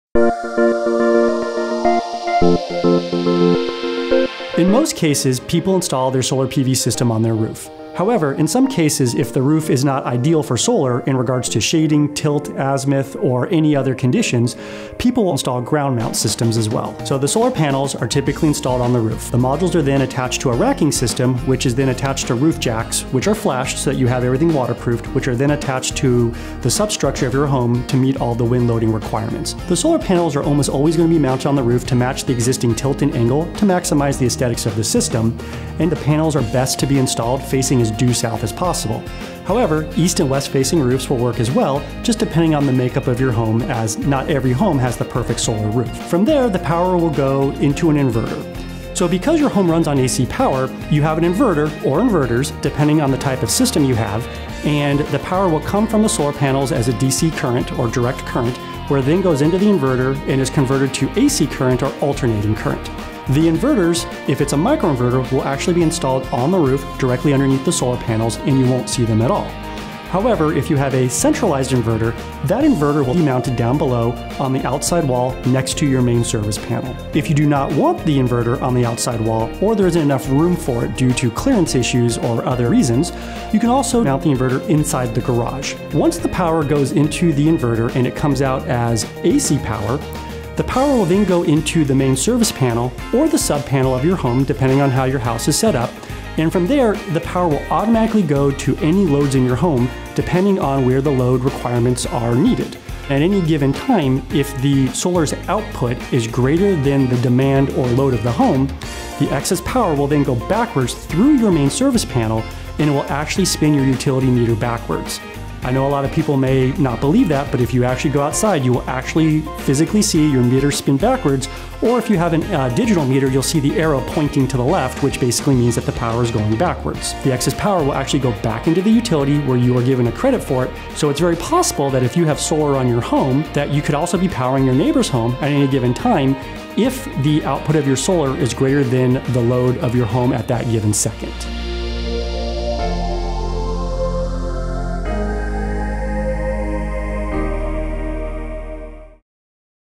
How Does My Solar System Work? | Solar FAQs
We’re here to answer all of your solar questions!

From the moment the photovoltaic process begins, a series of actions are carried out by your system to make sure that the energy from the Sun can actually power your house.

From the photovoltaic process to power being converted through inverters to it going through your utility grid, Semper Solaris' solar expert and Executive Director of Operations, Robert Bessler, explains how the entire system works — it is our job to make sure you are on the loop and help you understand all things solar!